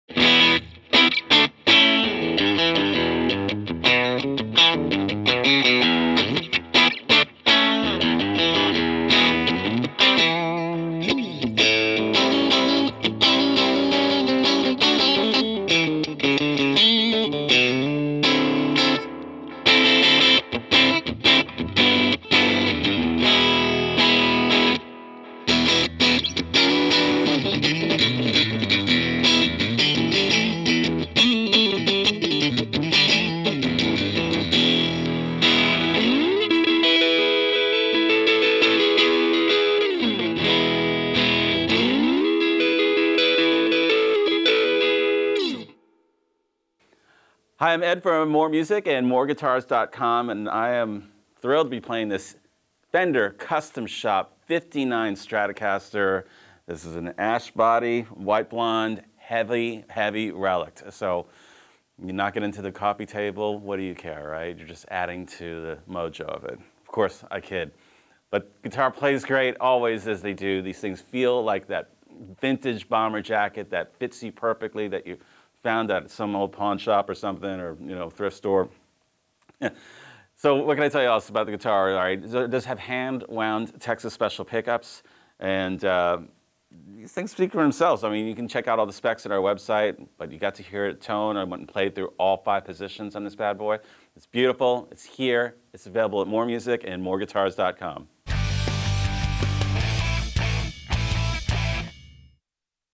Fender Custom Shop '59 Strat - Comfy to Play, but the Tone!
Ed's rhythmic mastery and these hand-wound Texas Special pickups are made for each other. Most player's who've had the chance to play an actual '59 Strat marvel at the "modern" feel of the neck carve. They're shredder guitars. This Fender Custom Shop creation nails the feel of the real deal.
Ash body, tasteful aging/relic'ing, vintage mod wiring, and hand-wound TS pickups. An authentic '59 recreation down to the string trees.
This is the one.